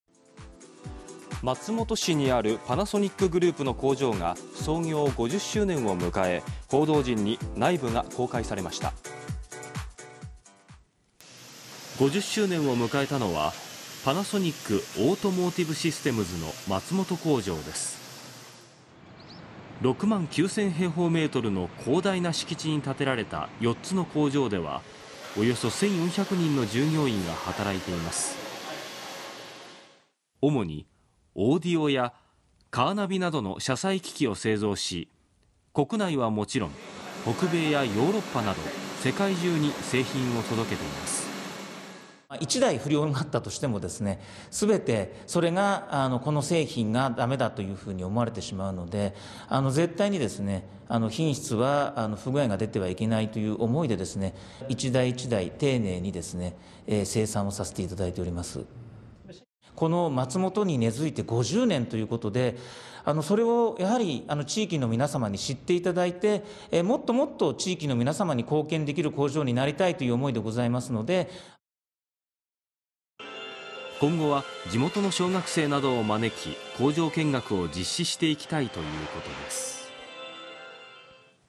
松本市のパナソニック工場が50周年！報道陣に工場内部を初公開（abnステーション 2024.11.12）
松本市にあるパナソニックグループの工場が操業50周年を迎え、報道陣に工場の内部が公開されました。

50周年を迎えたのは、「パナソニックオートモーティブシステムズ」の松本工場です。
6万9000平方メートルの広大な敷地に建てられた4つの工場では、およそ1400人の従業員が働いています。

主にオーディオやカーナビなどの車載機器を製造し、国内はもちろん北米や欧州などの世界中に製品を届けています。

■パナソニックオートモーティブシステムズ松本工場・粟澤学 工場長
「1台に不良があったとしたら、すべてこの製品は駄目だと思われてしまうので、絶対に品質は不具合が出てはいけないという思いで、一台一台丁寧に生産している。松本に根付いて50年ということで、地域の皆様に知って頂いて、もっと地域に貢献できる工場になりたい」

今後は、地元の小学生などを招き、工場見学を実施していきたいということです。